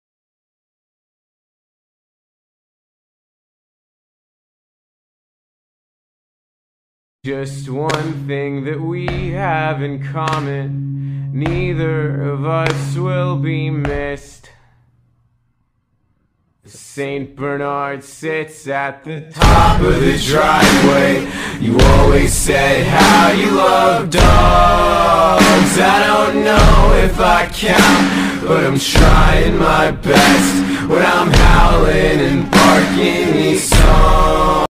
neither of us will be missed!||meme||gacha club||{tmnt Mikey's Au's} read description
so in this video, we see Sci for the first time in his adventures in different Au's, failing to save one of the Mikey.